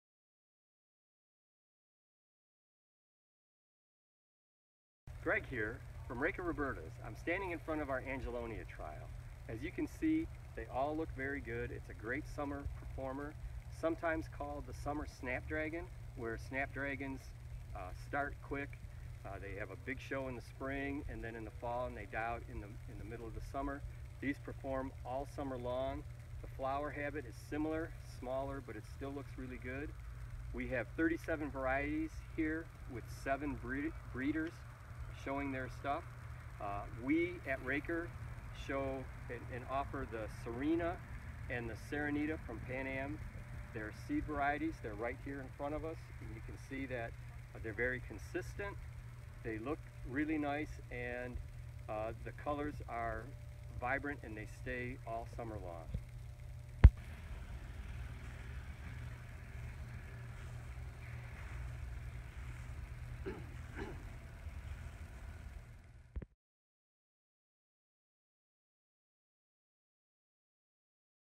2020 R2 Angelonia
Greg talks about the Raker-Roberta's Angelonia trial.